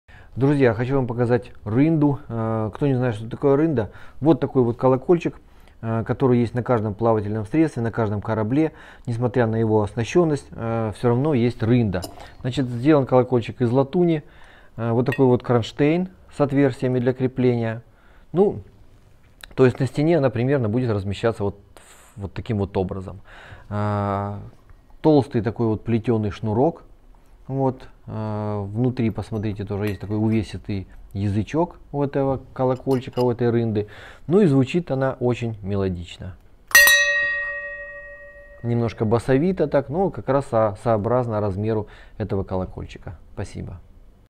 Рында корабельная
Корабли постоянно меняются и прошли длинную эволюцию: от утлого парусника до атомного ледокола. Не изменилась за это время только рында корабельная.